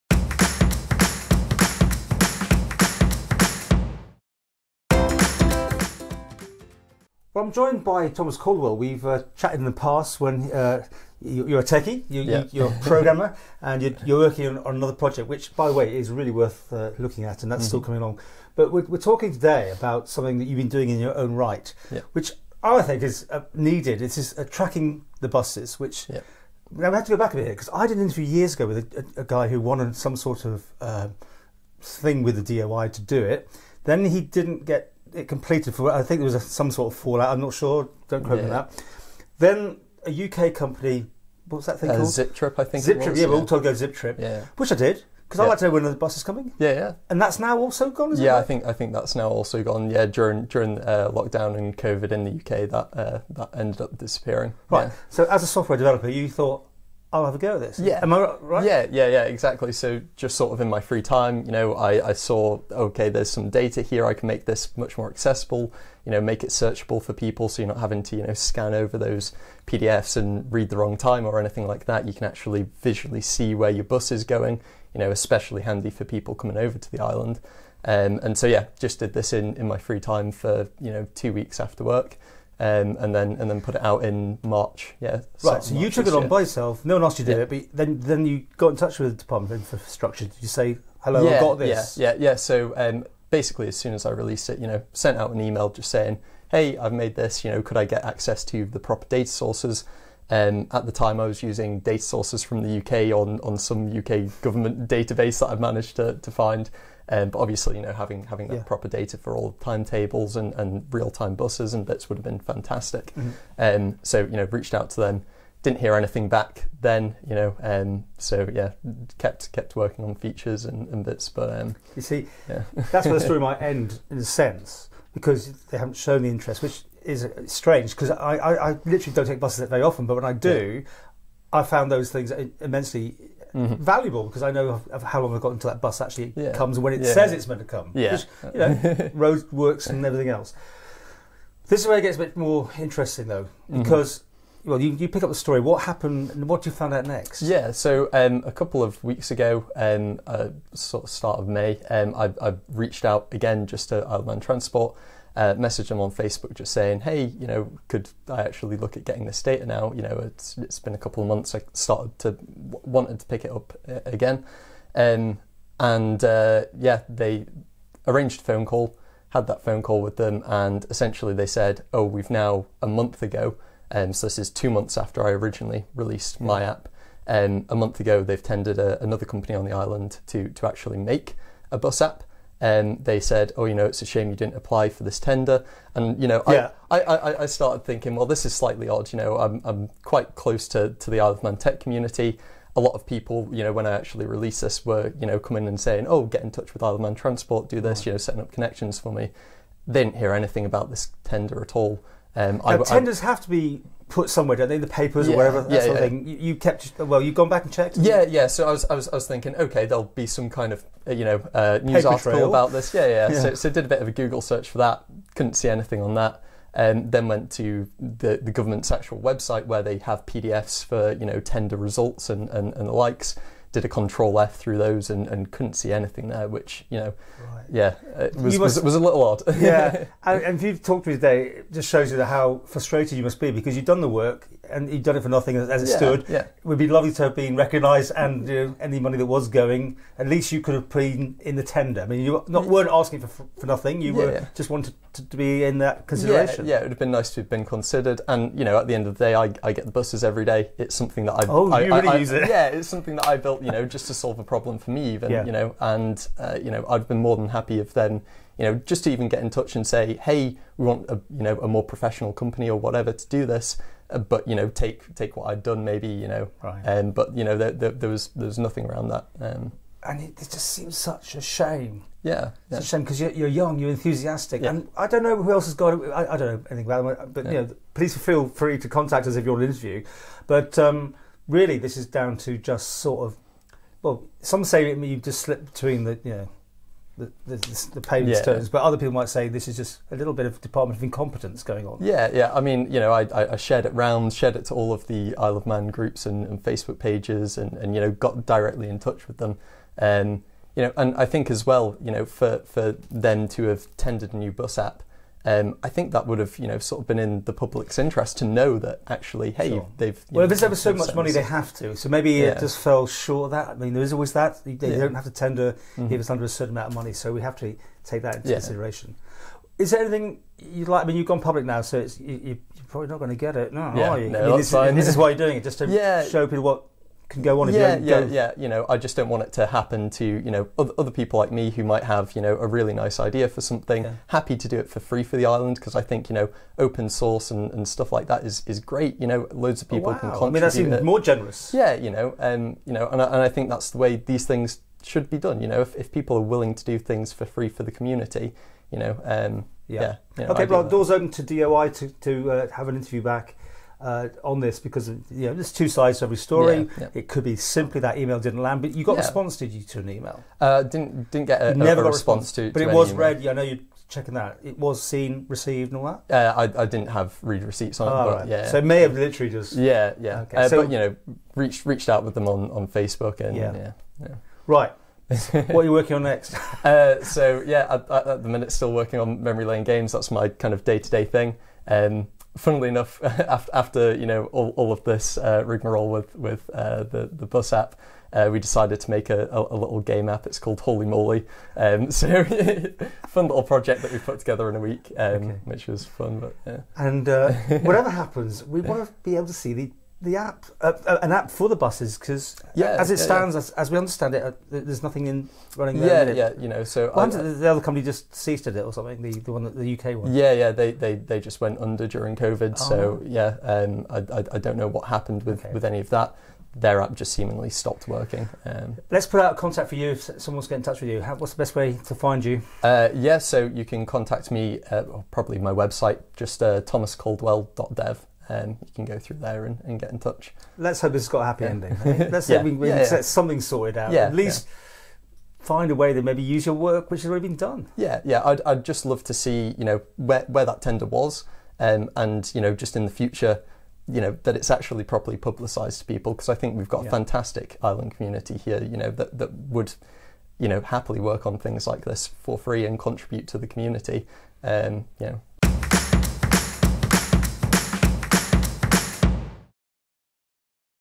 Software developer’s bus app
Thomas Coldwell is a local software developer, who has made a new app in his own time, to track the Bus Vannin network. He tells Paul Moulton his frustrating story in trying to get the Manx Government interested in using it...for free.